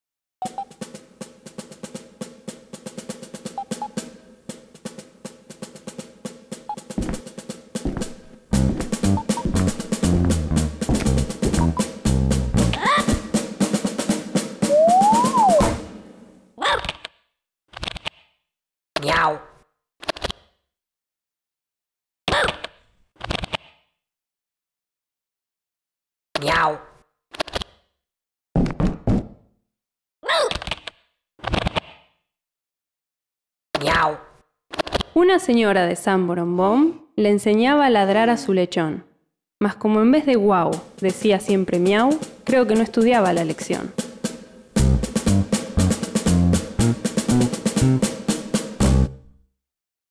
15 LECHÓN María Elena Walsh-LIMERICKS
Siguiendo una forma poética breve, de tradición anglosajona, María Elena Walsh creó disparatados y divertidos limericks para su libro Zooloco. A través de ilustración animada y locución, cada verso cobra vida propia a puro ritmo y rima.